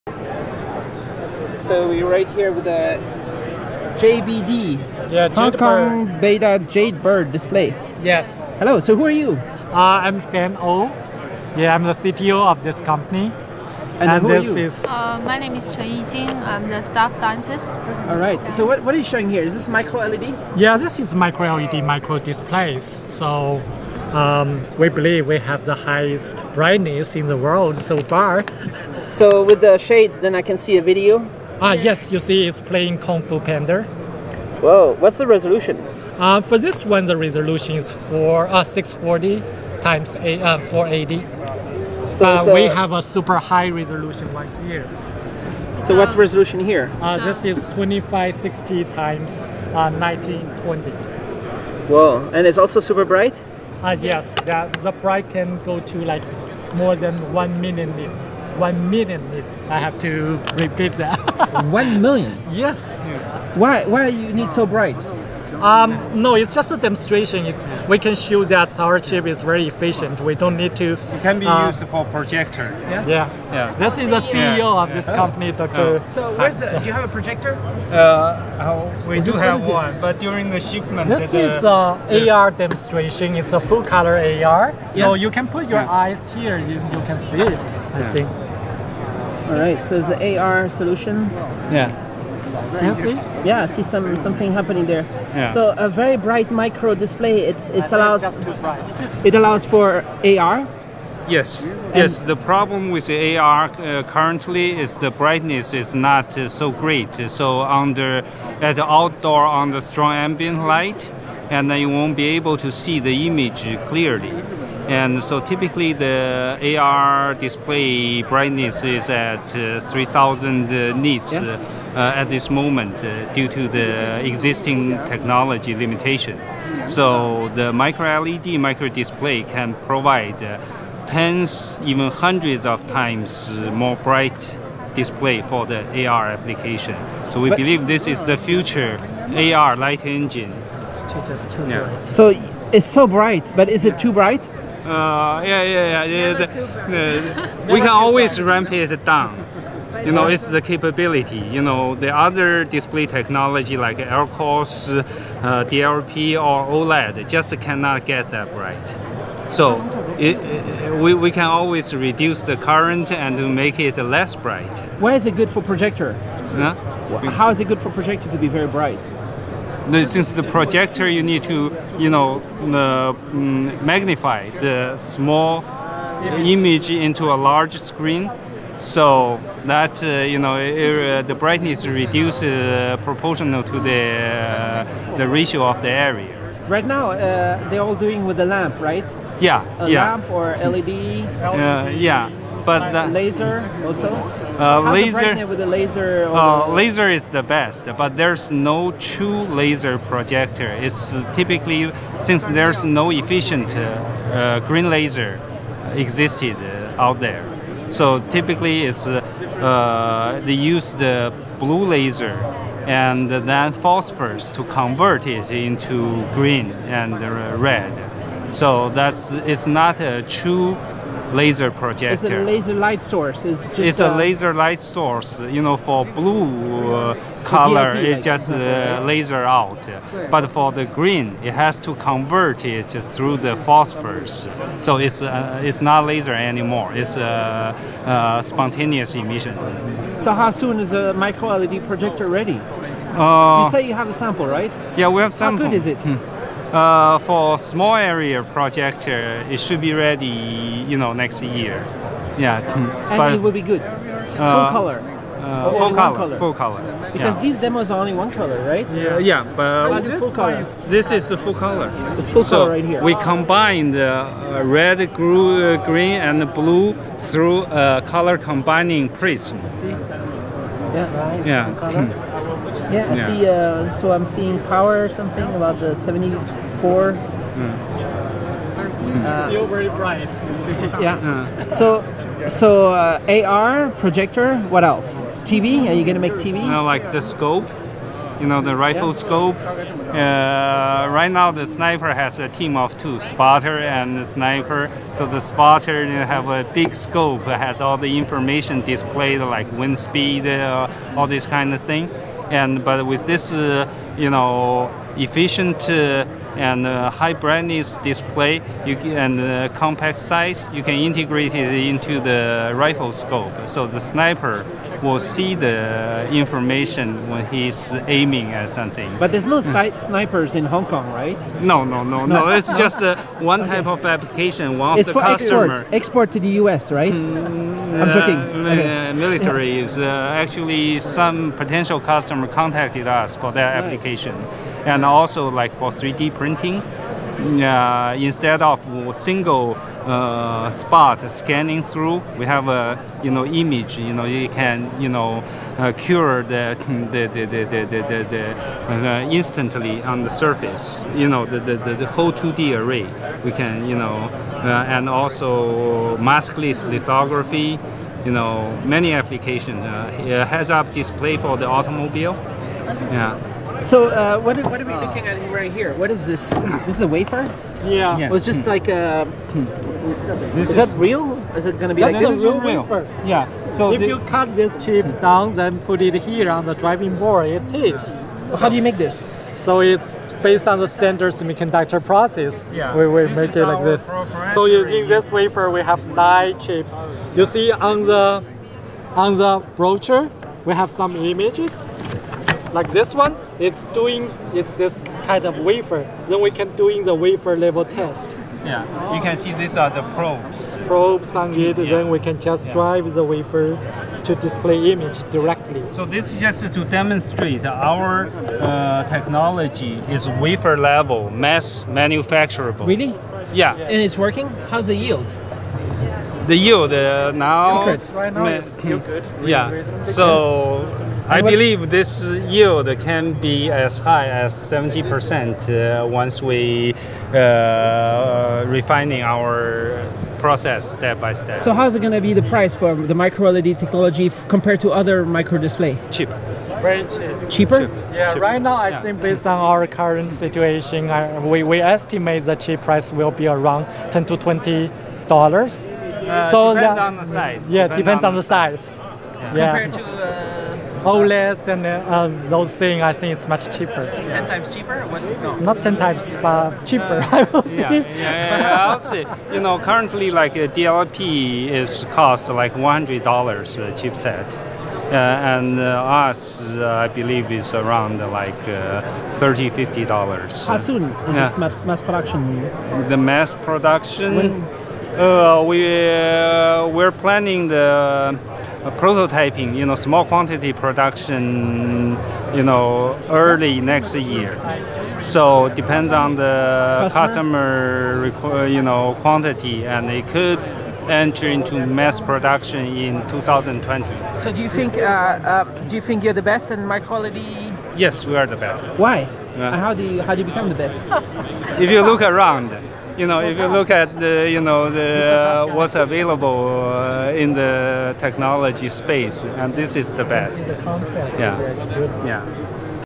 microLED, 5000ppi, brightest display in the world (1 million nits) by Jade Bird Display (JBD)
Hong Kong Beida Jade Bird Display (JBD) received an honorable mention from I-Zone judges for its active-matrix microLED display with 5,000 pixels per inch and over 1 million nits of brightness. JBD develops next-gen inorganic material-based microLED microdisplays using its unique wafer-scale, monolithic hybrid-integration technology, which allows the excellent light emission of compound semiconductor devices to be paired with IC functionality. JBD’s AMOLED microdisplays provide a solution for applications in augmented reality and other projection formats.

Filmed at the I-Zone demo and prototype area at SID Display Week, the world's largest and best exhibition for electronic information display technology.